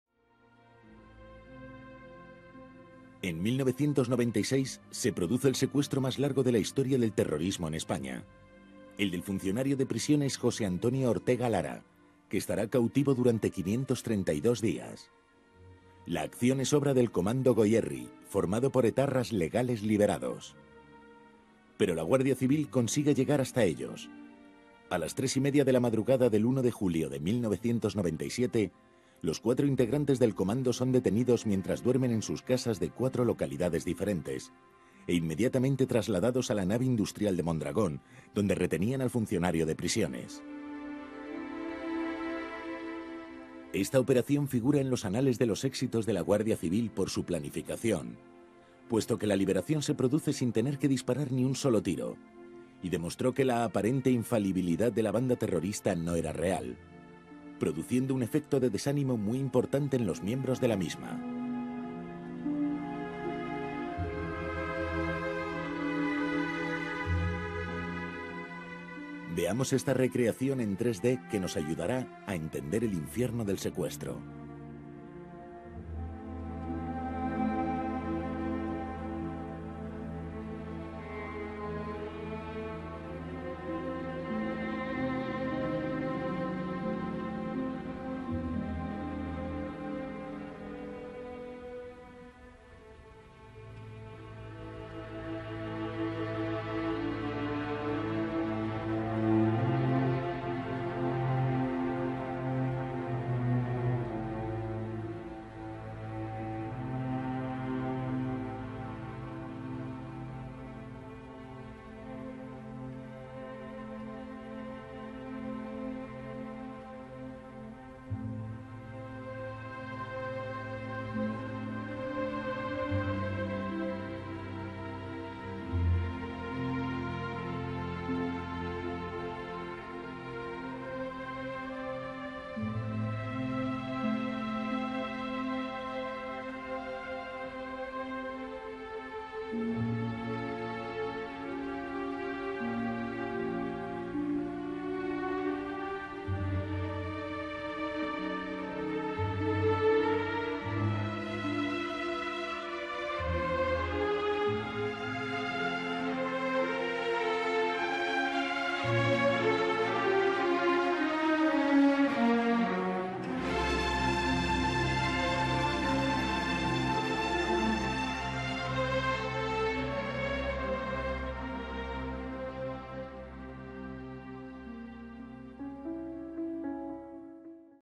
El zulo de Ortega Lara
Las imágenes que vais a ver las obtuve en la exposición "La Guardia Civil, escudo de la democracia frente al terrorismo", que en enero de 2012 se celebró en Pamplona.
Gracias, amigos. Como vosotros, chocamos las manos y damos saltos de alegría.
Todos los detalles en Desolvidar.